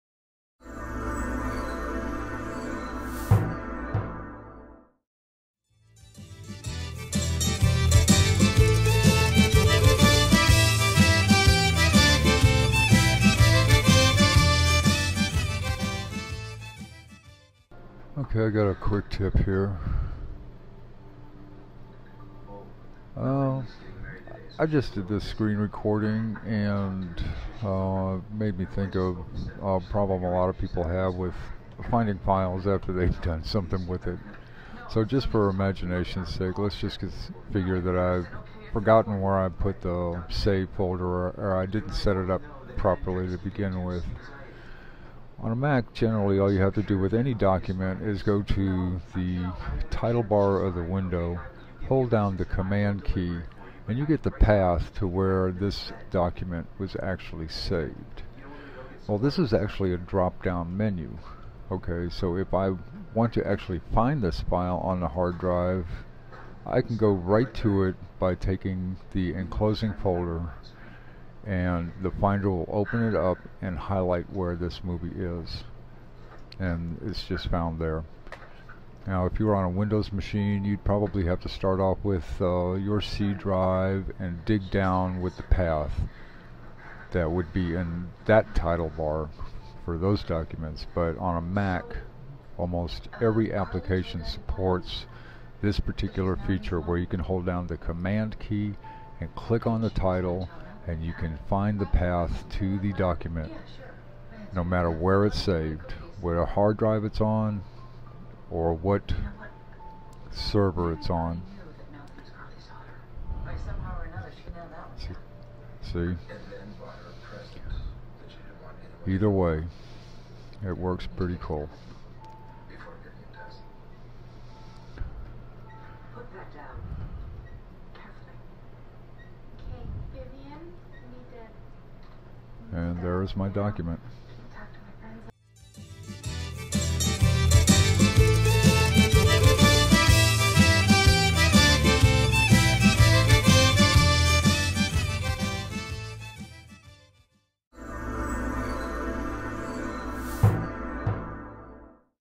Feature Tip - OS X ; unscripted
First tutorial

A quick tip about a feature in the Mac's OS that applies to most documents that will help you navigate to documents from *most* Mac applications (notable exceptions are Garageband and Final Cut.)

This feature is apparently "little known" even to a lot of long-time Mac users.

What I didn't say in the video:  the same feature is available in Finder windows as well, which helps out considerably in navigating UP the path chain without having to start at the top of the disk and work back down.  This feature is very useful when looking for *related* files that are currently not open in the application -- you won't need to minimize your work to get into the Finder since it will automatically be brought to the front when an enclosing directory is selected from the dropdown menu.

Hopefully this tutorial will be the first of many to come, concentrating, most likely on Photoshop, with a mixture of Motion, Final Cut; and with Logic for the audio side.